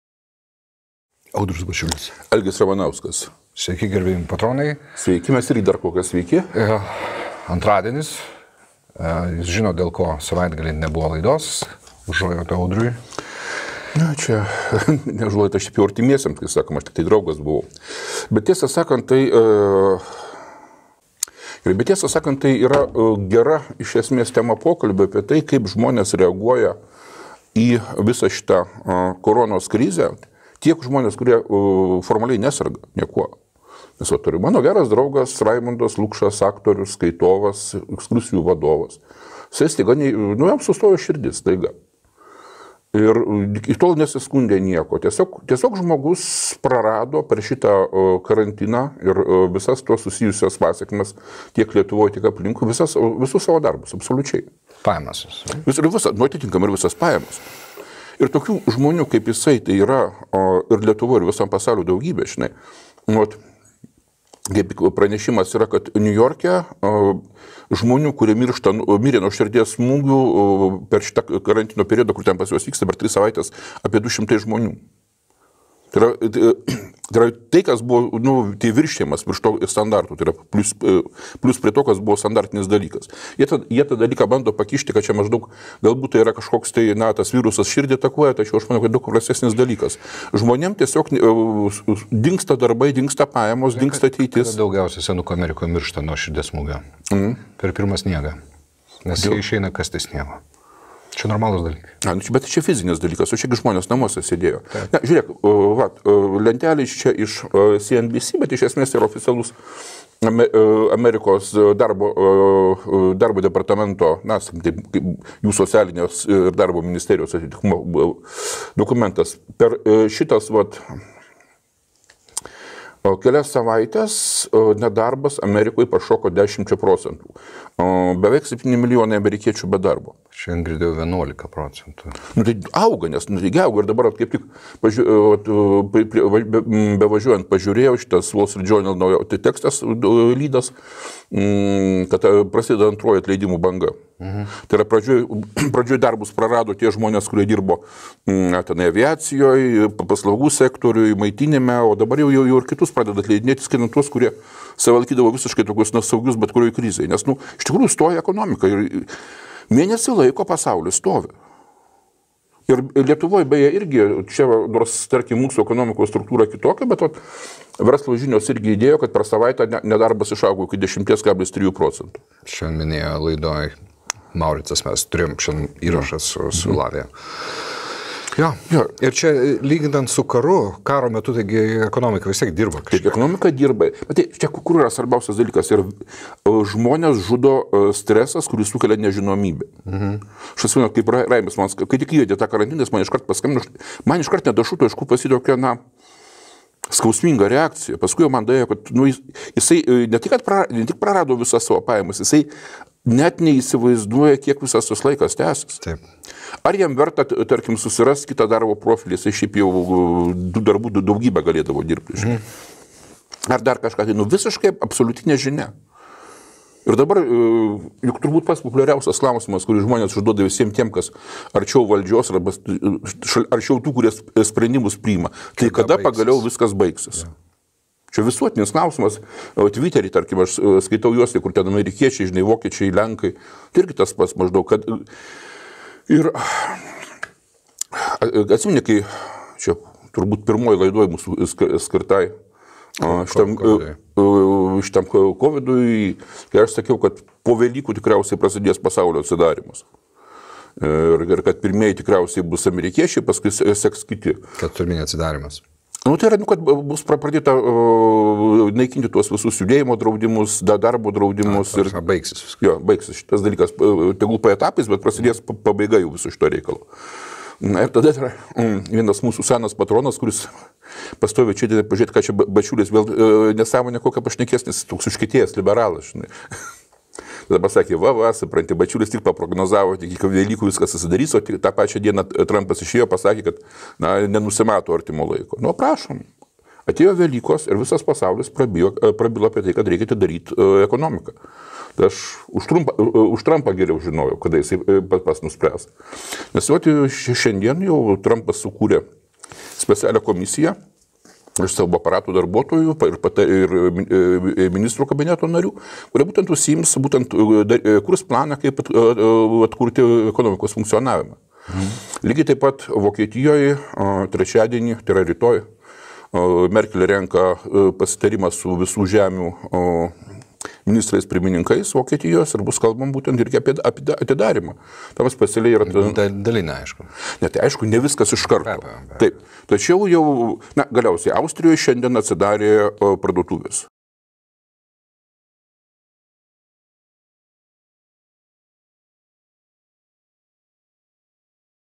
B&R S02E32 Karantinas į pabaigą? | Rusiją | Putin ir COVID | JAV kuria integruotą gynybą IŠTRAUKA
PILNO EPIZODO
TEMOS:

Karantinas eina į pabaigą?
Saudai prispaudė Rusiją 00:18:30
Putin ir COVID 00:25:56
JAV kuria integruotą oro gynybą 00:52:02